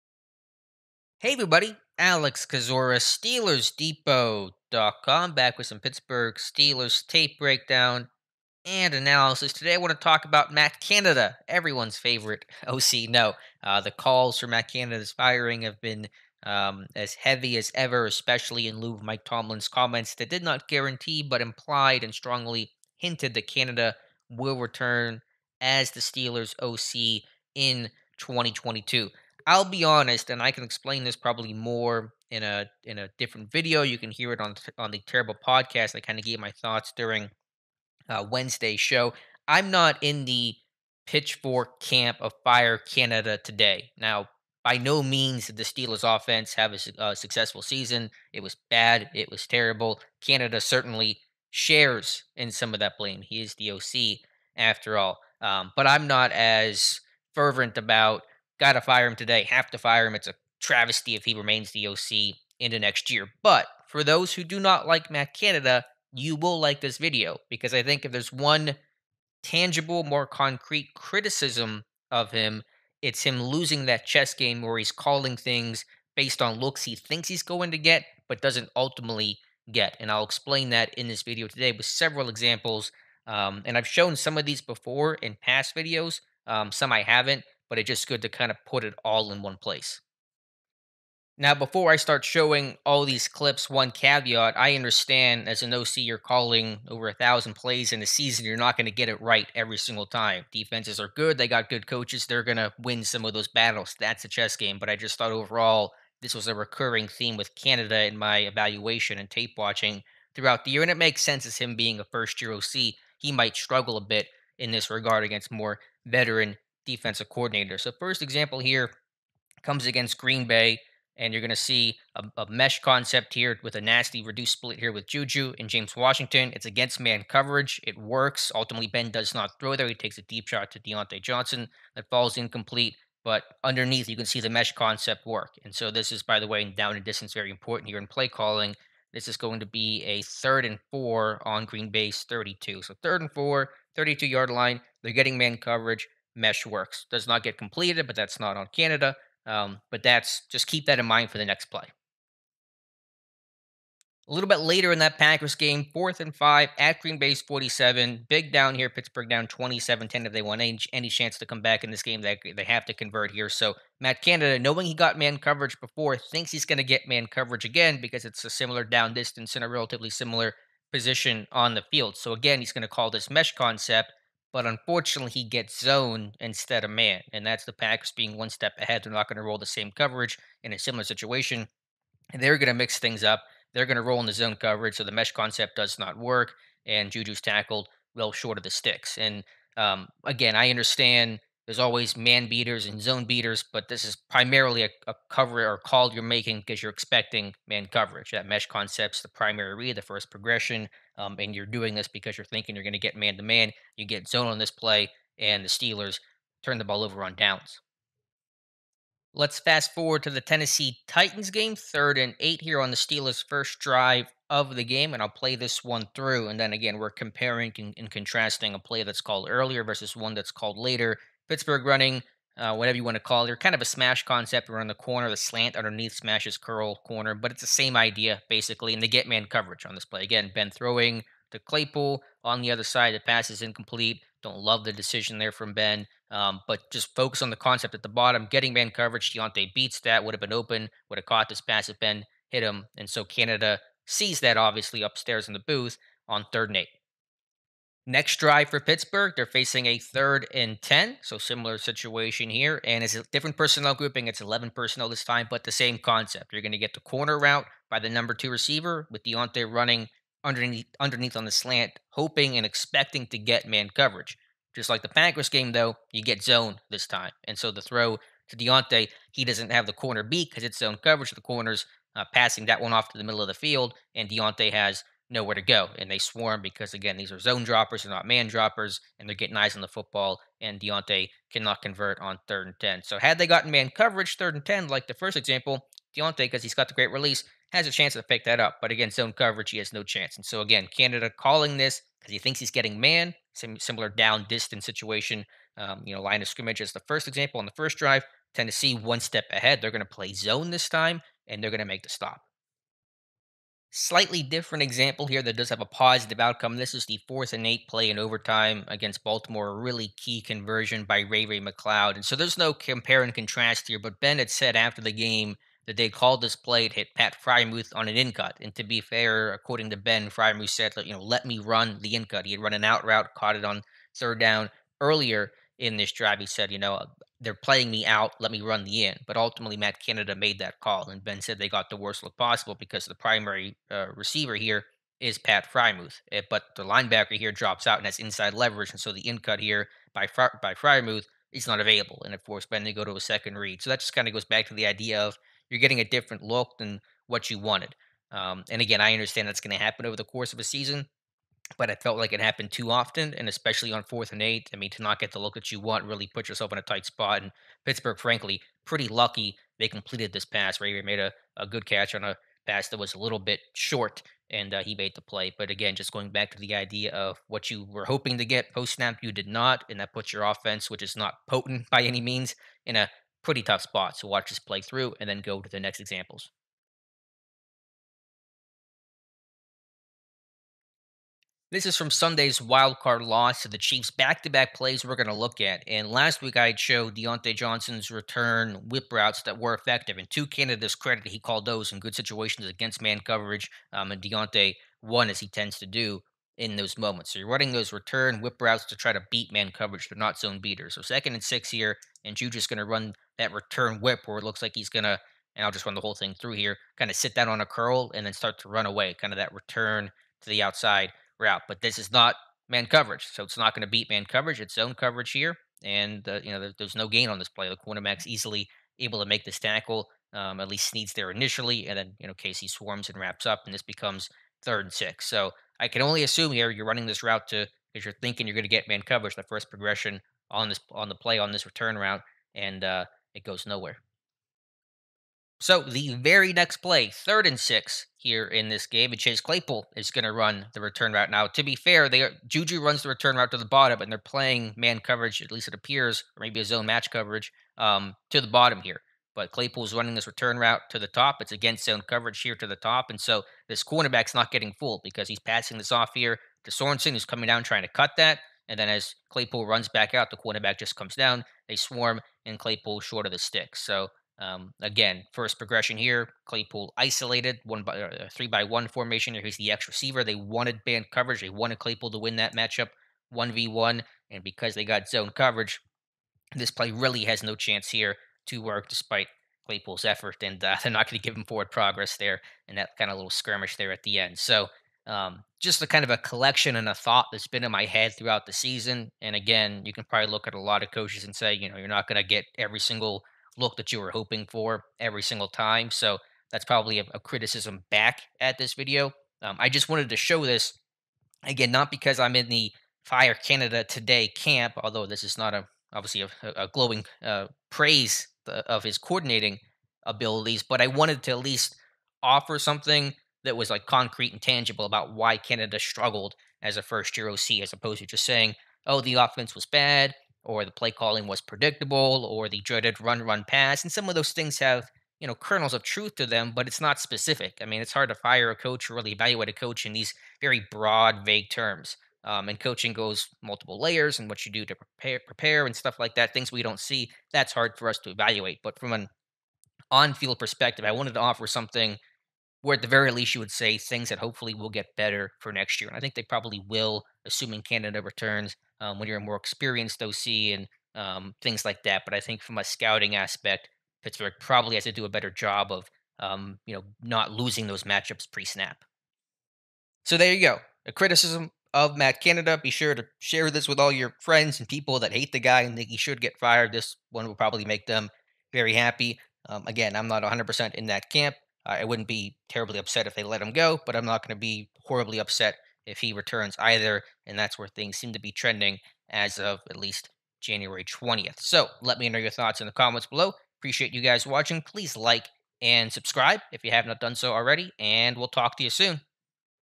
Film Room: A Legitimate Criticism Of Matt Canada
Breaking down one of Matt Canada's weaknesses as Pittsburgh Steelers' offensive coordinator.